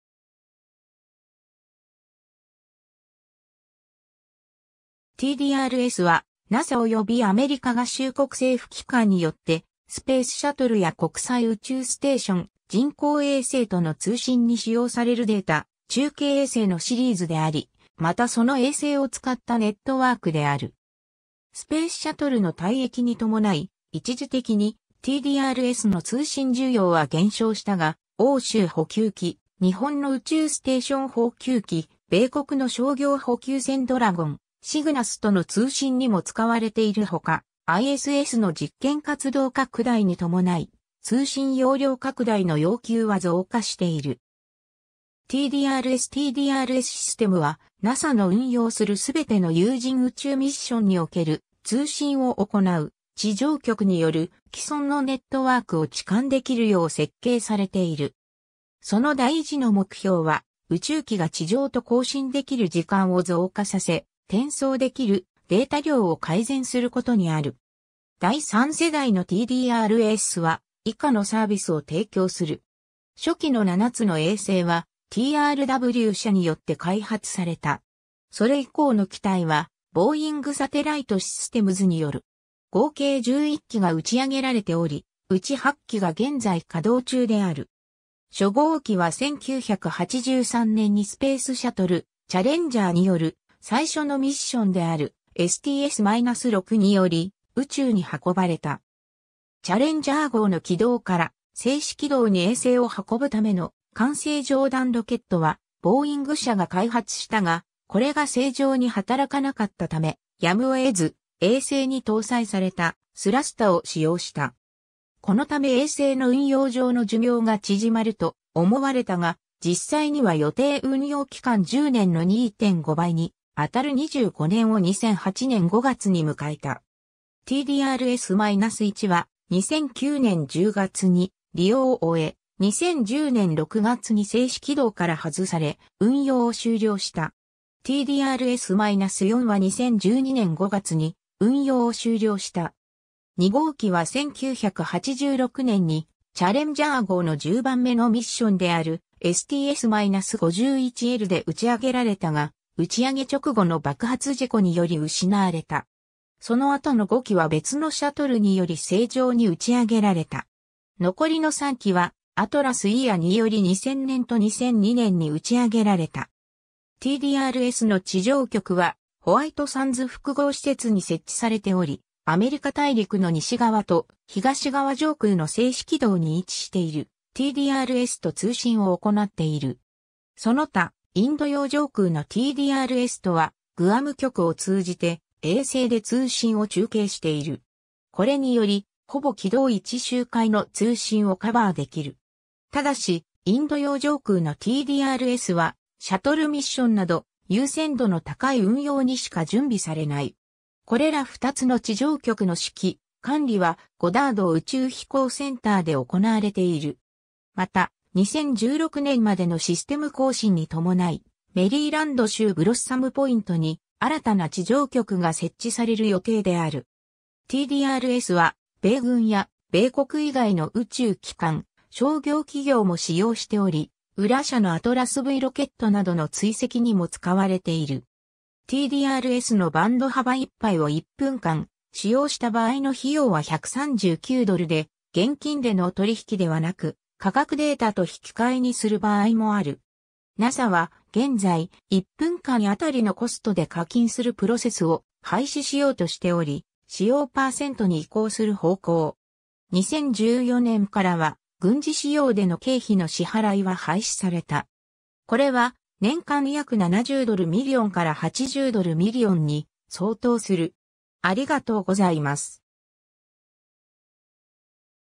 TDRS
TDRS (Tracking and Data Relay Satellite、追跡・データ中継衛星) は、NASAおよびアメリカ合衆国政府機関によって、スペースシャトルや国際宇宙ステーション (ISS)、人工衛星 (ハッブル宇宙望遠鏡、ランドサット、TRMM、EOS、NASAの多数の天体観測衛星など)との通信に使用されるデータ中継衛星のシリーズであり、またその衛星を使ったネットワークである。
 スペースシャトルの退役に伴い、一時的にTDRSの通信需要は減少したが、欧州補給機(ATV)、日本の宇宙ステーション補給機(HTV)、米国の商業補給船ドラゴン、シグナスとの通信にも使われているほか、ISSの実験活動拡大に伴い通信容量拡大の要求は増加している。
 TDRS（第1世代） TDRS（第2世代） システムはNASAの運用する全ての有人宇宙ミッションにおける通信をおこなう地上局による既存のネットワークを置換できるよう設計されている。
その第一の目標は、宇宙機が地上と交信できる時間を増加させ、転送できるデータ量を改善することにある。
第3世代のTDRSは以下のサービスを提供する。
 初期の7つの衛星はTRW社によって開発された。
それ以降の機体はボーイング・サテライト・システムズによる。
合計11機が打ち上げられており、うち8機が現在稼働中である。
 初号機は1983年にスペースシャトル「チャレンジャー」による最初のミッションであるSTS-6により宇宙に運ばれた。
チャレンジャー号の軌道から静止軌道に衛星を運ぶための慣性上段ロケットはボーイング社が開発したが、これが正常に働かなかったため、やむを得ず衛星に搭載されたスラスタを使用した。
このため衛星の運用上の寿命が縮まると思われたが、実際には予定運用期間10年の2.5倍にあたる25年を2008年5月に迎えた。
TDRS-1は、2009年10月に利用を終え、2010年6月に静止軌道から外され運用を終了した。
TDRS-4は2012年5月に運用を終了した。
 二号機は1986年にチャレンジャー号の10番目のミッションであるSTS-51-Lで打ち上げられたが、打ち上げ直後の爆発事故により失われた。
その後の5機は別のシャトルにより正常に打ち上げられた。
残りの3機はアトラスIIAにより2000年と2002年に打ち上げられた。
 TDRSの地上局は、ホワイトサンズ複合施設に設置されており、アメリカ大陸の西側と東側上空の静止軌道に位置しているTDRSと通信を行っている。
その他、インド洋上空のTDRSとはグアム局を通じて衛星で通信を中継している。
これにより、ほぼ軌道1周回の通信をカバーできる。
ただし、インド洋上空のTDRSはシャトルミッションなど、優先度の高い運用にしか準備されない。
これら２つの地上局の指揮/管理はゴダード宇宙飛行センターで行われている。
また、2016年までのシステム更新に伴い、メリーランド州ブロッサムポイントに新たな地上局が設置される予定である。
 TDRSは米軍や、米国以外の宇宙機関、商業企業も使用しており、ULA社のアトラスVロケットなどの追跡にも使われている。
 TDRSのバンド幅いっぱい(すなわち300Mbps)を1分間使用した場合の費用は$139で、現金での取引ではなく、科学データと引き換えにする場合もある。
NASAは現在、1分間当たりのコストで課金するプロセスを廃止しようとしており、使用パーセントに移行する方向。
2014年からは軍事使用での経費の支払いは廃止された。
これは年間約$70 million から$80 millionに相当する。